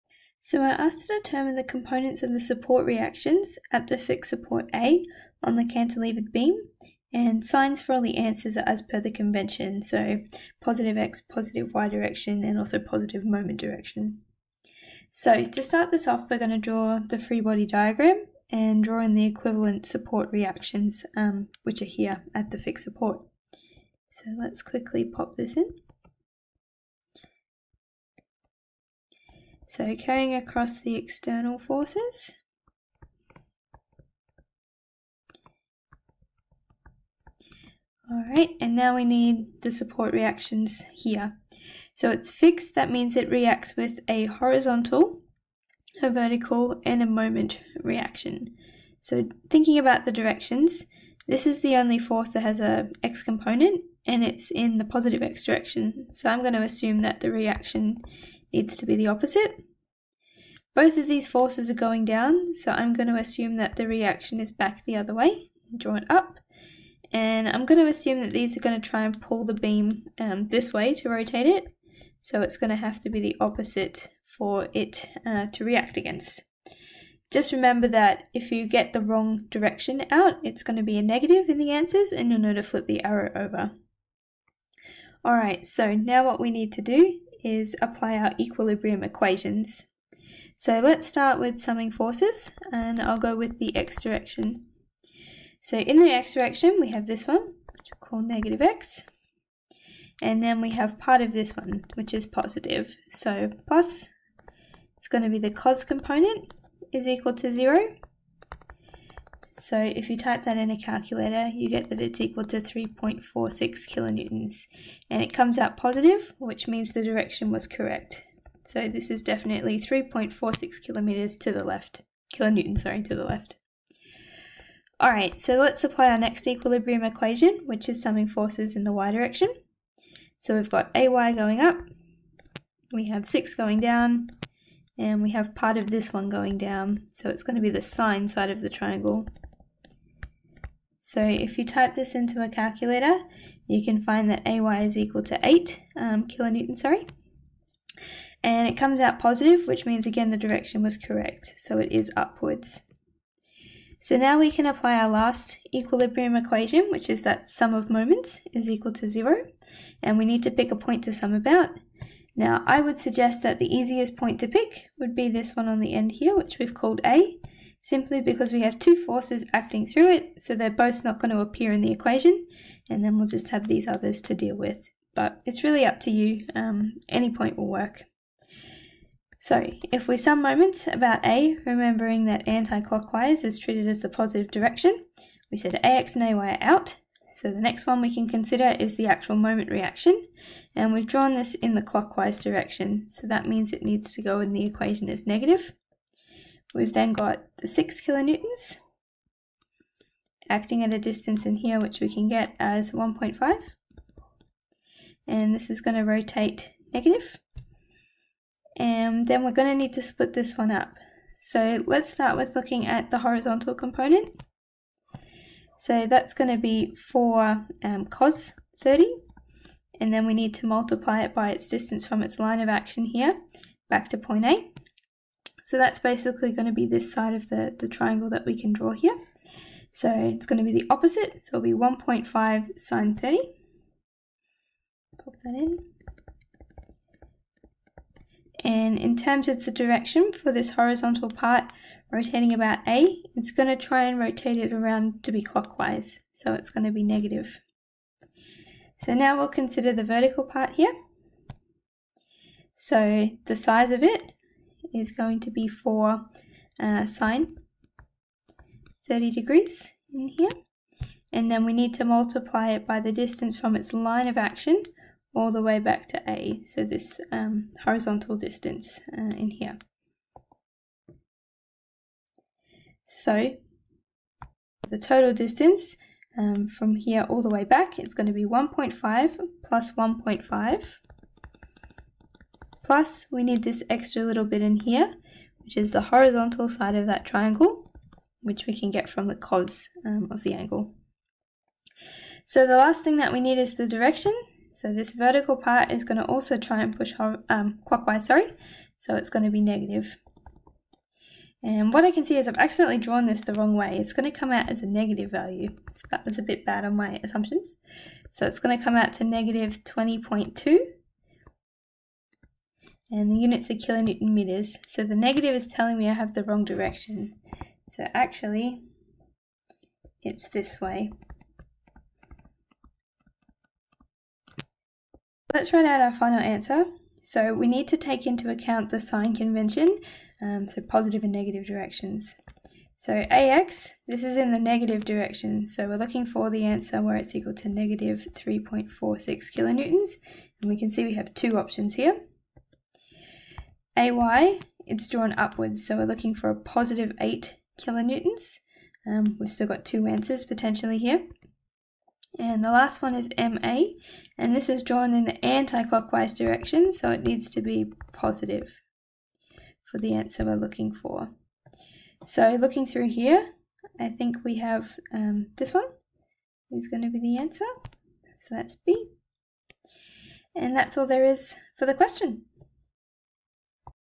Mechanics Sample Exam - Q6
This video solves for the support reactions of a static structure which has a fixed support connection to the wall. This is done using the equilibrium equations.